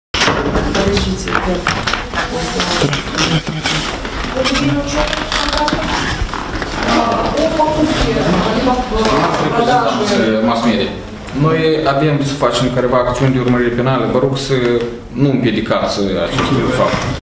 Представители Центра антикоррупции из Кишинева приехали рано утром в кабинет и.о. примара Бельц
Представители Центра антикоррупции из Кишинева (всего около 10 человек) приехали рано утром в кабинет и.о. примара Бельц Николая Григоришина (будущий кандидат в примары от НП), а потом рассосались по примарии. Изымаются документы и жёсткие диски компьютеров. Гостей интересует закупка контейнеров в период мусорного кризиса и земельные вопросы. Все это происходит на следующий же день после того как ЦИК, дождавшись указания Владимира Плахотнюка, назначил дату досрочных местных выборов.